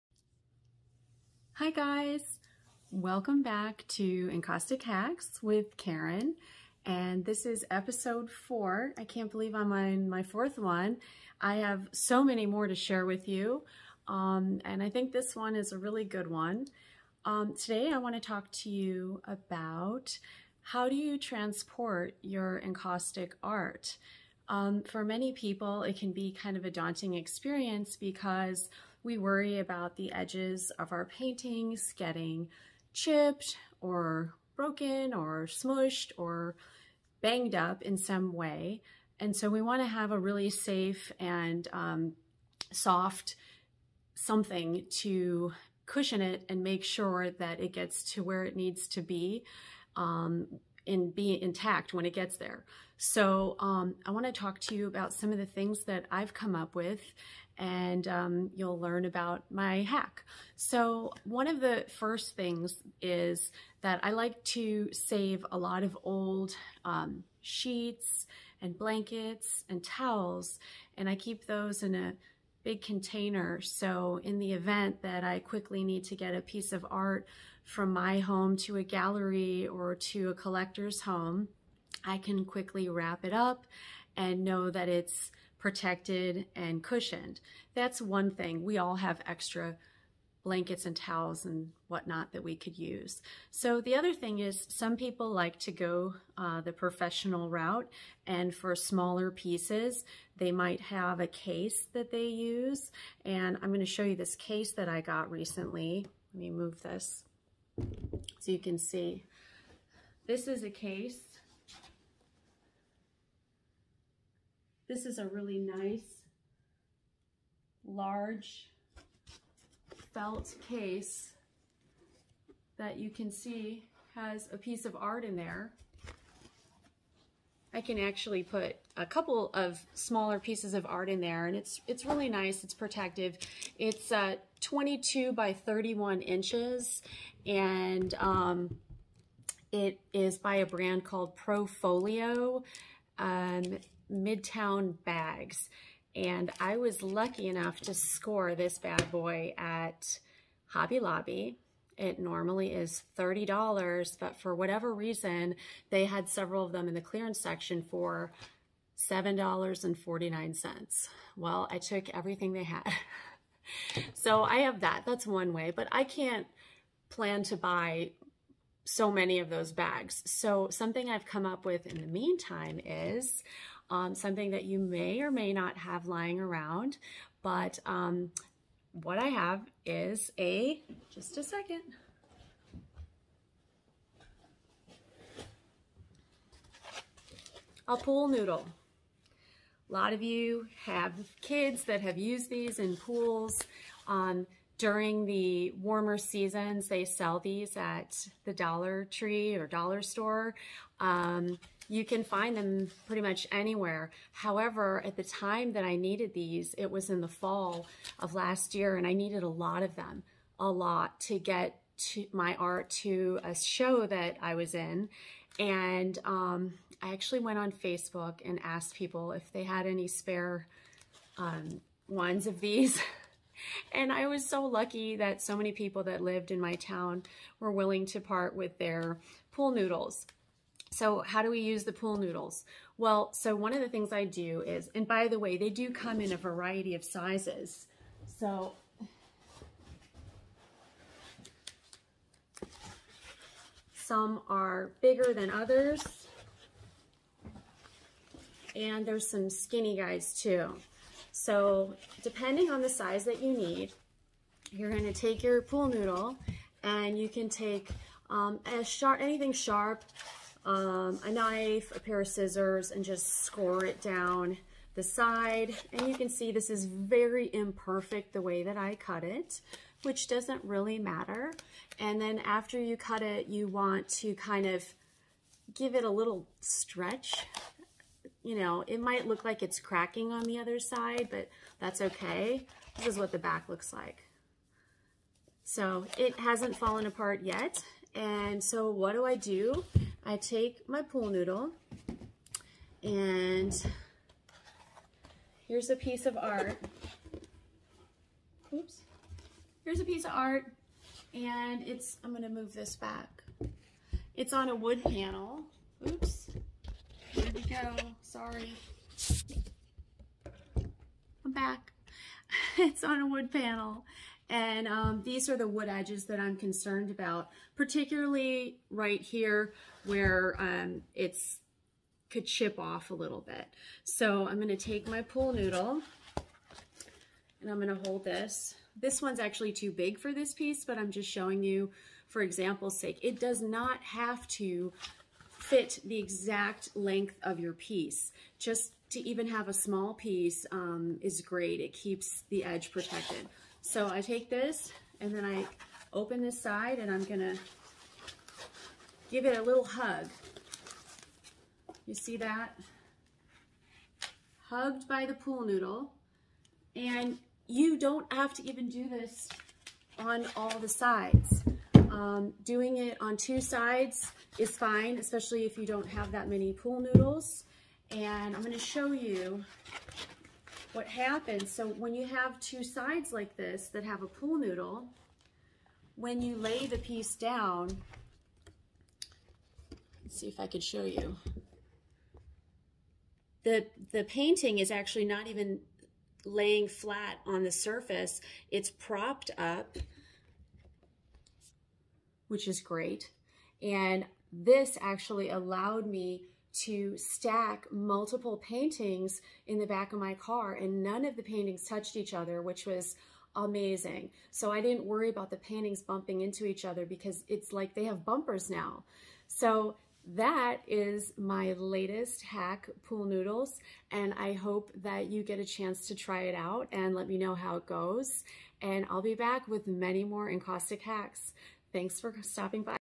Encaustic Art Hacks: Episode 4
Today we talk about ways to transport your encaustic work and keep it protected from damage. My hack is unexpected but I think you're gonna like it!